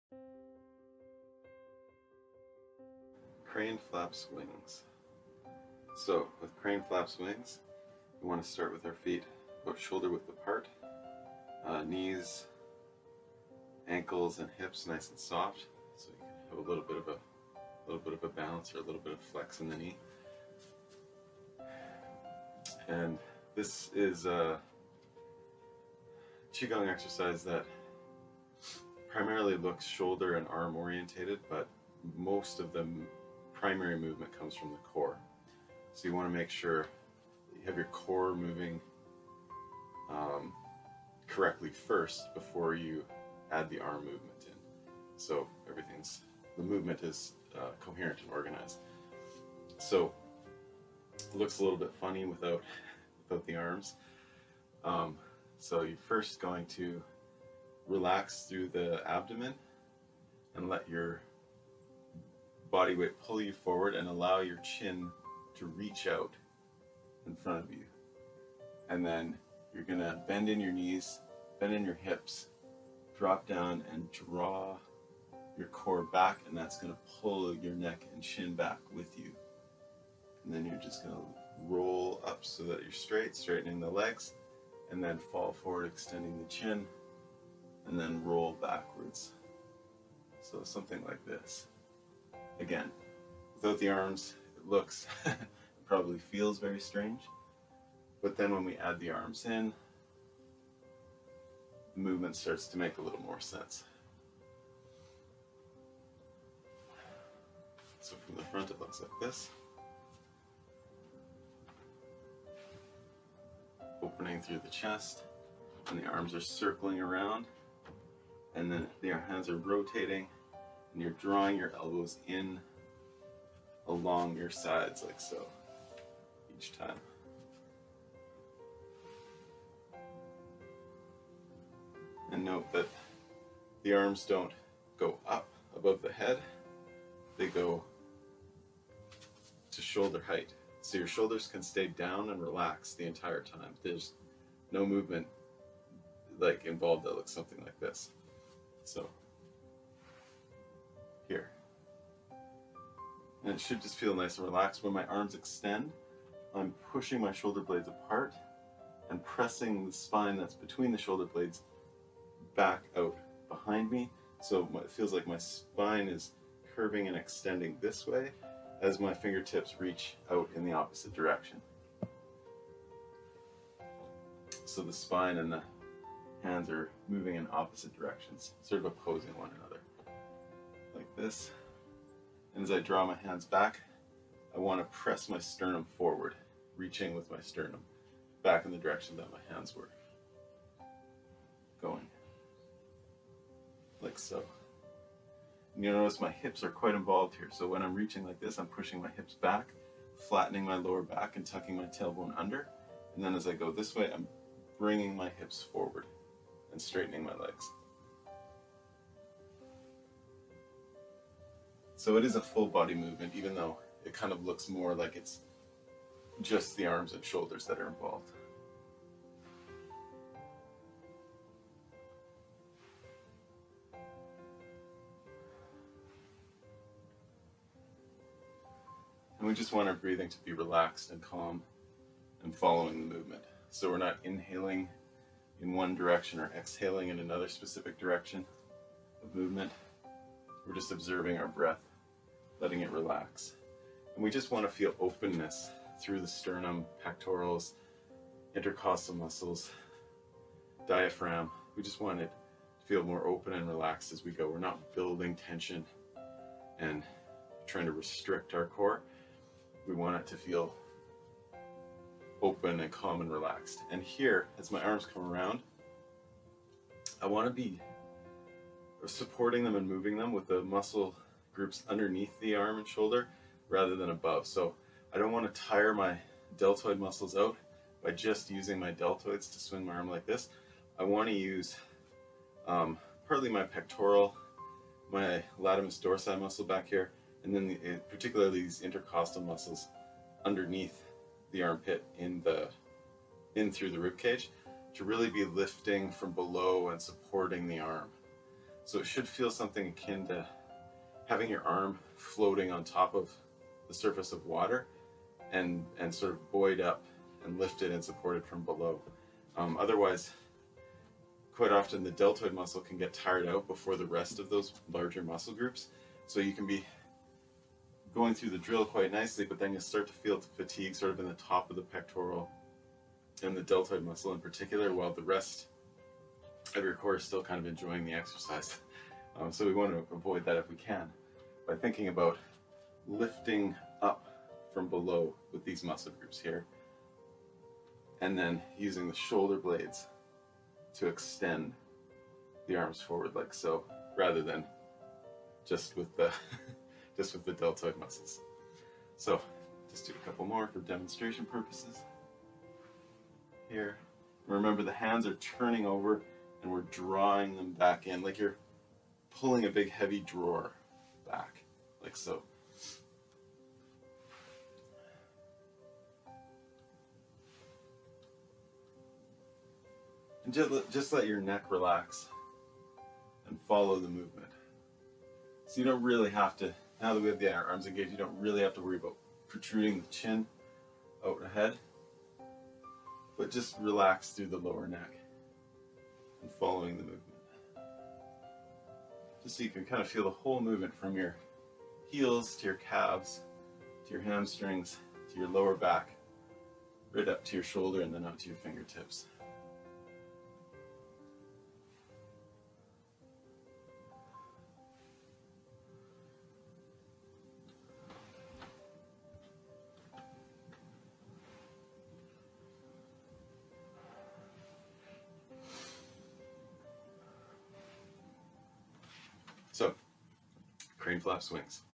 Chi Gung Demo # 3 Crane Flaps Wings
A quick overview of the 'Crane Flaps Wings' Chi Gung Exercise
--Link to the 36th Chamber Tai Chi and Chi Gung Facebook Group---